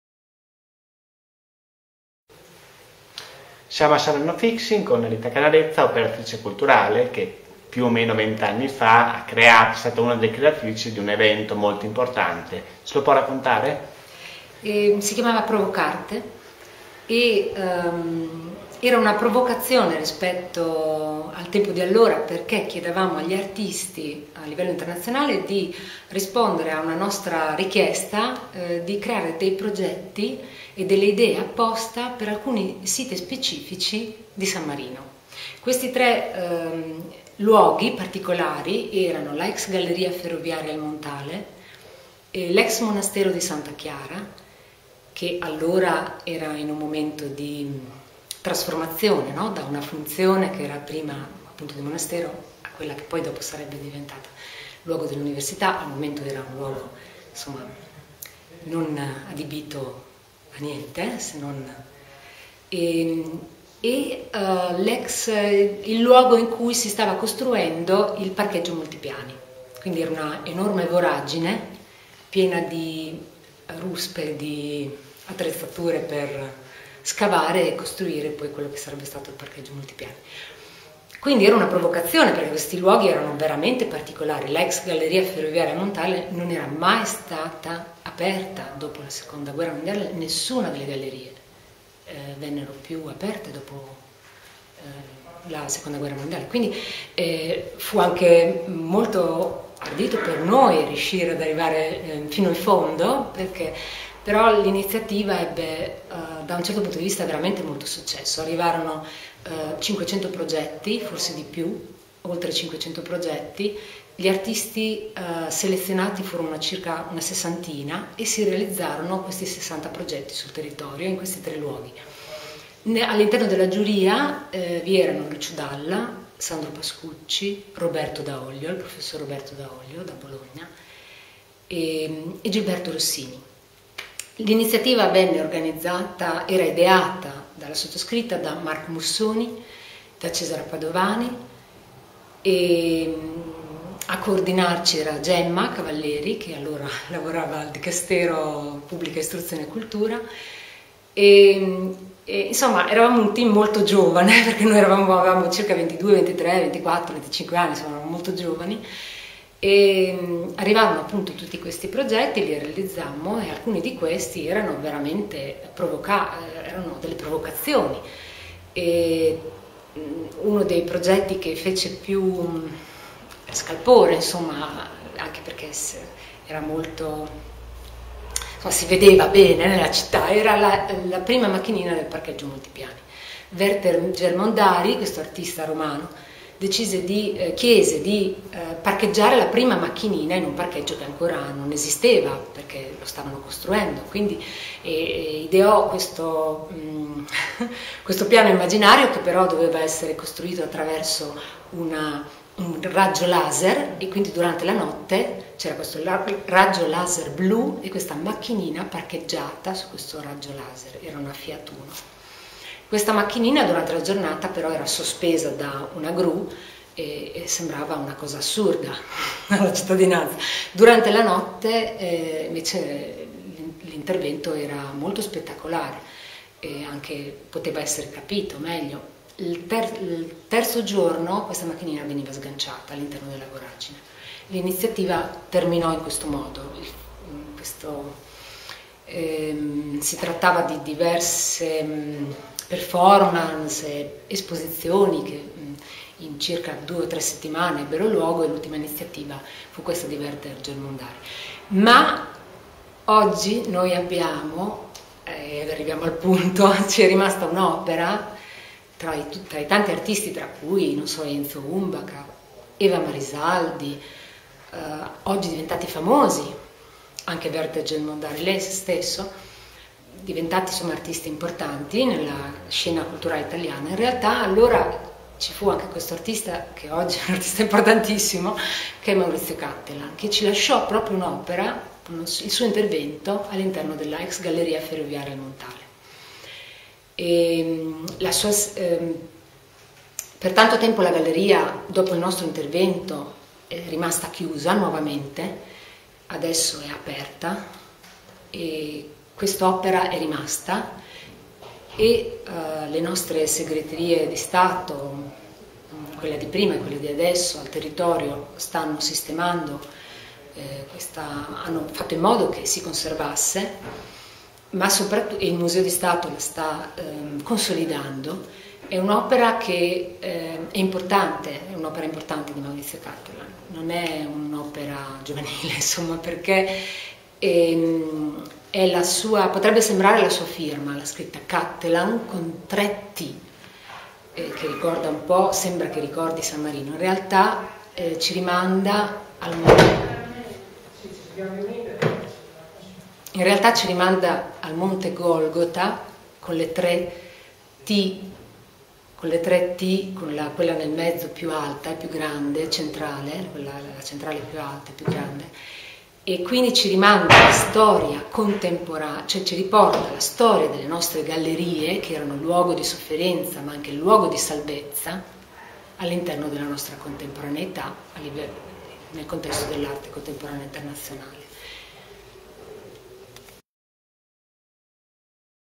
Rita Canarezza: Dalla e Cattelan
San Marino: l'operatrice culturale Rita Canarezza racconta "Provoc'arte", l'evento del 1991 che portò sul Titano anche Lucio Dalla e un giovanissimo Maurizio Cattelan. Cattelan lasciò una sua opera nella galleria "Il Montale": la sua firma con tre t.